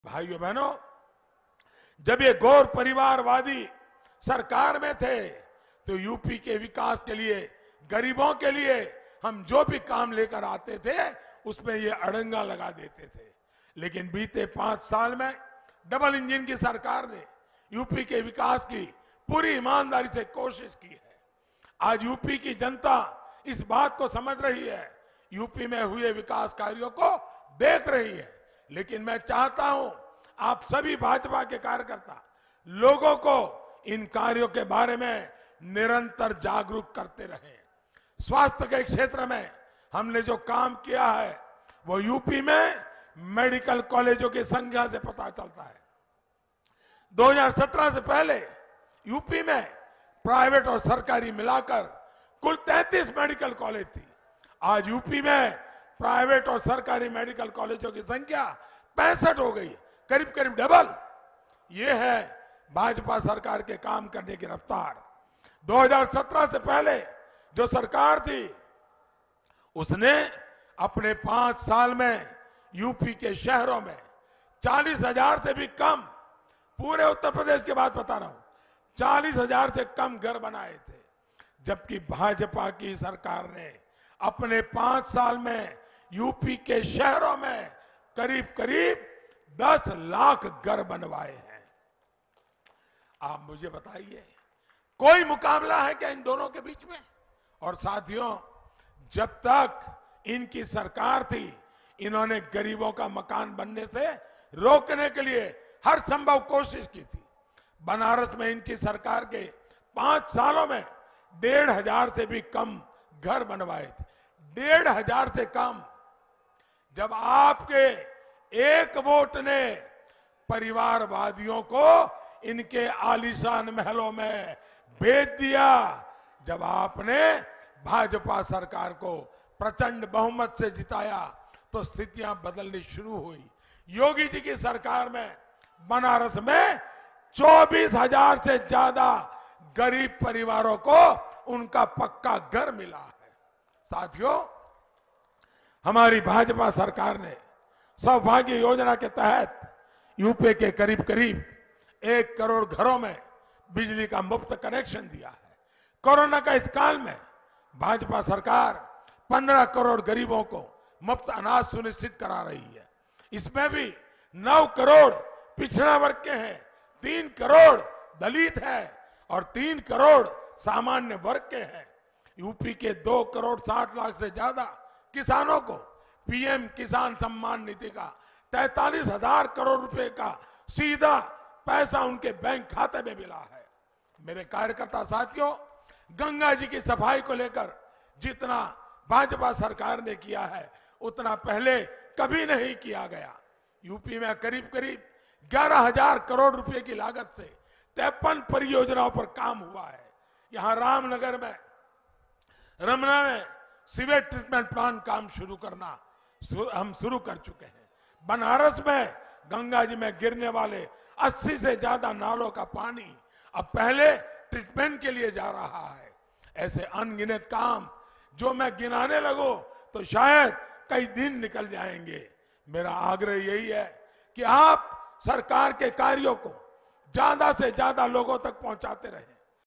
Urge every BJP Karyakarta to make people aware of BJP’s work: PM Modi
Prime Minister @NarendraModi today addressed a public meeting in Uttar Pradesh’s Varanasi. Addressing the huge gathering, PM Modi said, “The party which has the strength of hardworking karyakartas like you, then its victory is sure. This shows your enthusiasm and confidence in Booth Vijay Sammelan that we will win every booth as well, will score full marks in this entire region. And I know, BJP karyakartas do not rest till the last day of election. Whatever role the party gives, you put your life into it.”